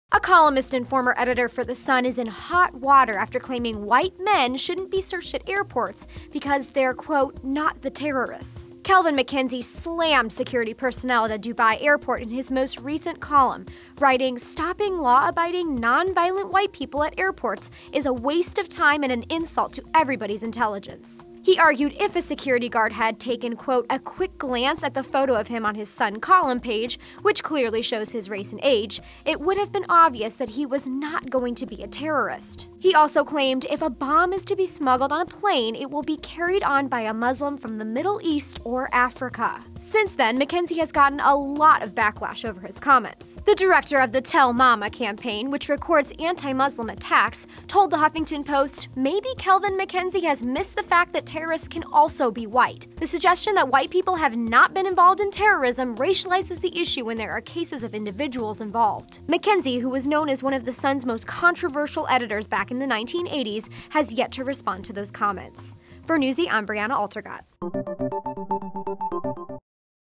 Sun Columnist Says White People Shouldn't Be Searched At Airports - Newsy
Kelvin MacKenzie used his regular column for The Sun to express his indignation that he, a 69-year-old white man, was searched by airport security.

Sources:
Getty Images